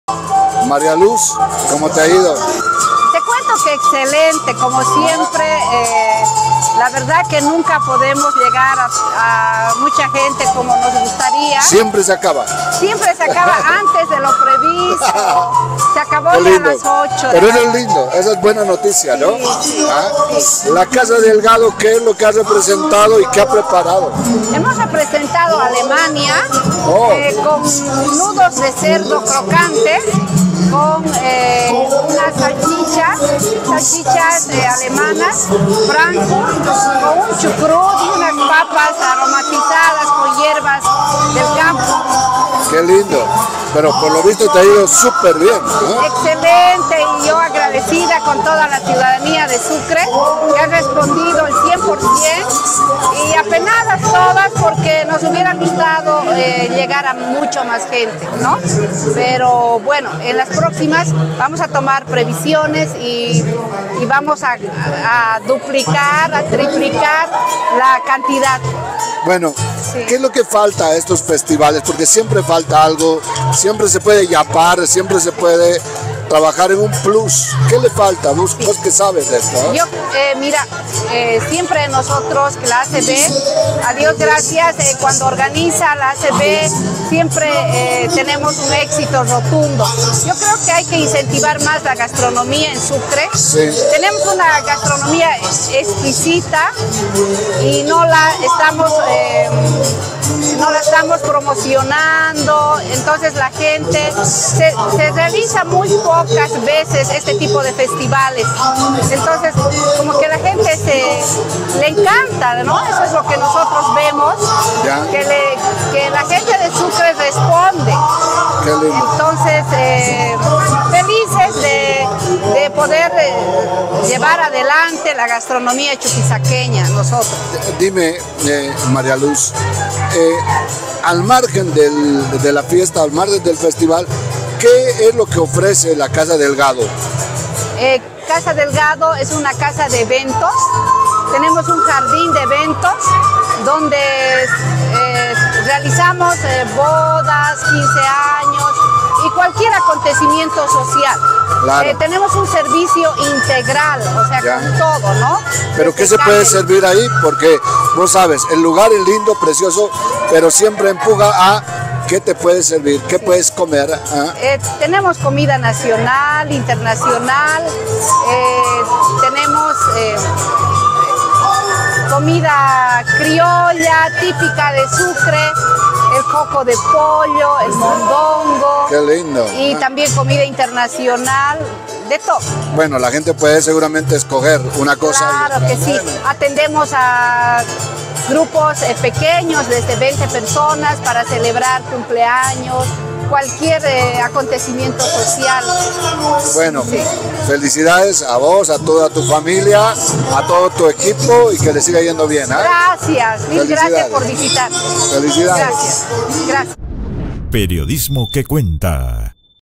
𝗟𝗔 “𝗖𝗔𝗦𝗔 𝗗𝗘𝗟𝗚𝗔𝗗𝗢”, 𝗟𝗜𝗗𝗘𝗥𝗔𝗗𝗔 𝗣𝗢𝗥 𝗠𝗔𝗥𝗜́𝗔 𝗟𝗨𝗭 𝗖𝗢𝗥𝗧𝗘𝗭, 𝗖𝗢𝗡𝗤𝗨𝗜𝗦𝗧𝗔 𝗟𝗢𝗦 𝗣𝗔𝗟𝗔𝗗𝗔𝗥𝗘𝗦 𝗘𝗡 '𝗨𝗡 𝗠𝗨𝗡𝗗𝗢 𝗗𝗘 𝗦𝗔𝗕𝗢𝗥𝗘𝗦
La renombrada chef María Luz Cortez cautivó a los comensales en el festival "Un Mundo de Sabores", celebrado el viernes 7 de julio en el majestuoso patio colonial de la facultad de Derecho de la Universidad San Francisco Xavier.

El evento reunió a amantes de la buena comida y la cultura culinaria, brindando a la talentosa chef el escenario perfecto para exhibir su maestría en la cocina.

La Casa Delgado, bajo la dirección de María Luz, presentó con gran éxito una exquisita selección de platos de la gastronomía alemana. Entre los destacados se encontraban el cerdo crocante, las salchichas con chucrut y las papas aromatizadas con hierbas del campo. Cada bocado era una delicia que logró satisfacer incluso a los paladares más exigentes.

La chef María Luz Cortez estuvo acompañada en este importante evento por su invaluable equipo, compuesto por su propia familia. Juntos, honraron el nombre de Casa Delgado y brindaron a los asistentes una experiencia culinaria inolvidable.

Durante el festival, María Luz compartió detalles sobre su emprendimiento culinario con los presentes. Además de su destacada participación en festivales gastronómicos, la chef ha sabido organizar eventos personalizados y de gran envergadura, manteniendo siempre altos estándares de calidad. Desde matrimonios hasta cumpleaños y actividades sociales, María Luz y su equipo se han consolidado como expertos en ofrecer experiencias gastronómicas únicas y memorables.

En cuanto a la oferta culinaria de La Casa Delgado, se destacaron platos típicos como el mondongo, el ckocko de pollo, el picante mixto y los chorizos, entre una amplia variedad de sabores y opciones para satisfacer todos los gustos.

En resumen, la participación de la chef María Luz Cortez y su equipo en el festival "Un Mundo de Sabores" fue un rotundo éxito. Su destacada propuesta gastronómica alemana deleitó a los asistentes, quienes tuvieron la oportunidad de disfrutar de una experiencia culinaria excepcional en el majestuoso entorno de la Universidad San Francisco Xavier.

María Luz y su Casa Delgado se mantienen como referentes indiscutibles en el mundo de la alta cocina.